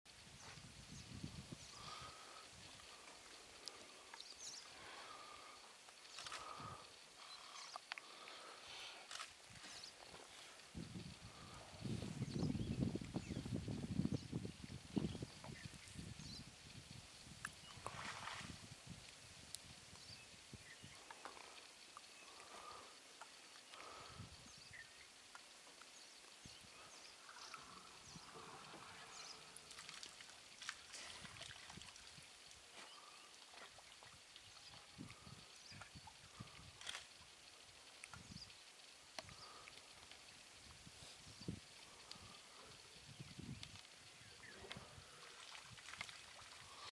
Elephant Drinking In The Winter Sun
Musth Bull Elephant takes a sip of water.